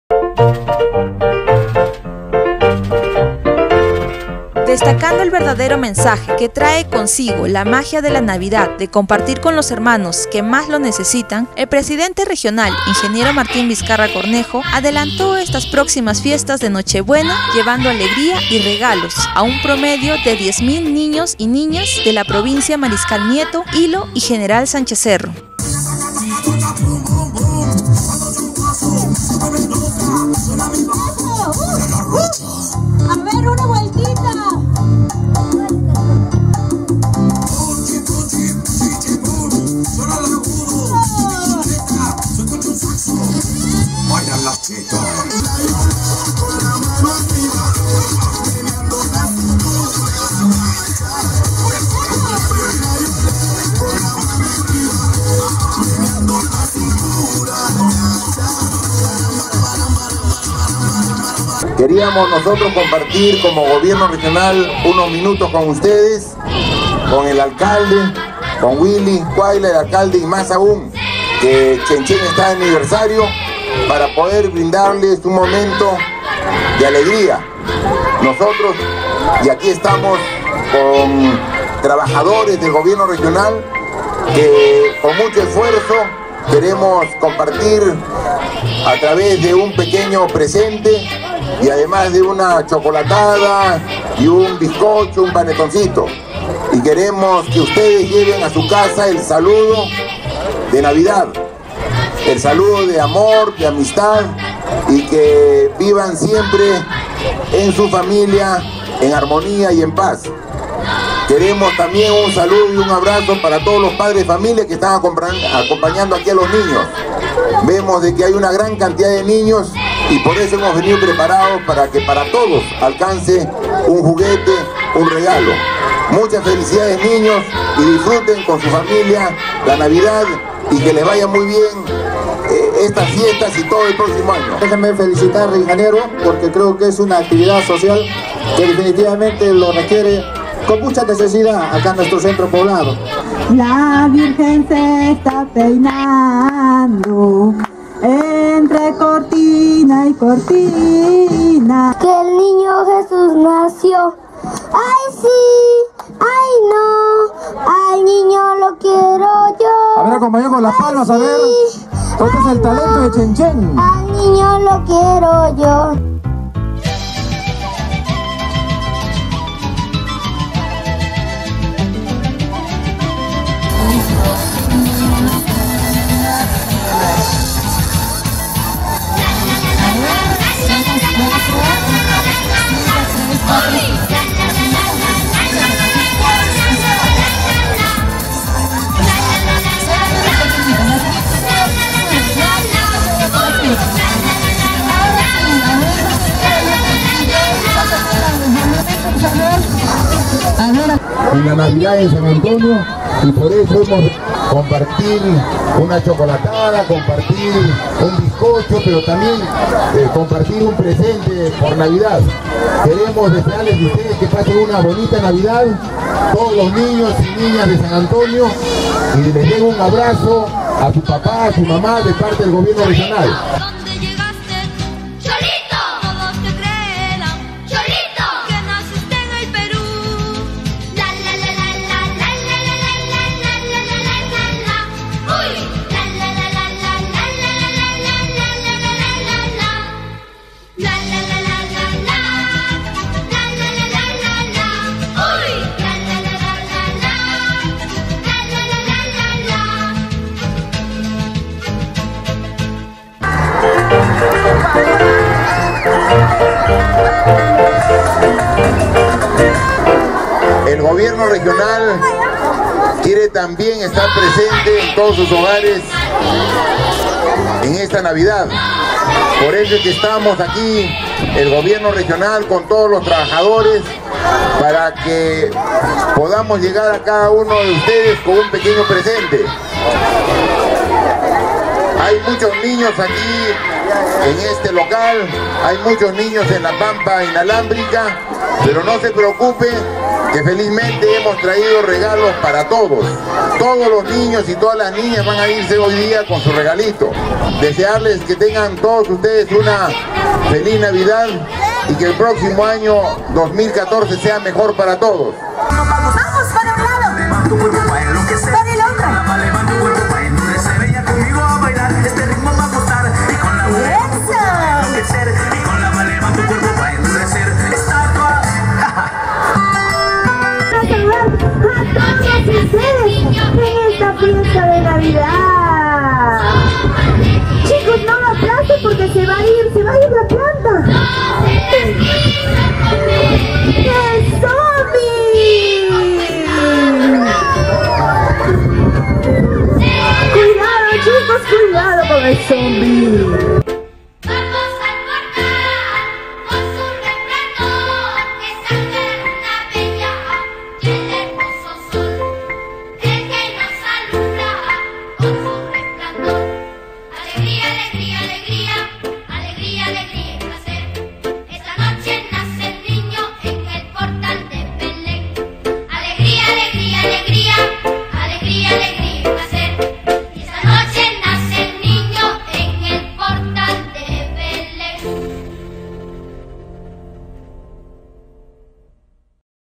Feliz Navidad GRM
PRESIDENTE REGIONAL ADELANTÓ NAVIDAD LLEVANDO ALEGRÍA Y REGALOS A NIÑOS y NIÑOS.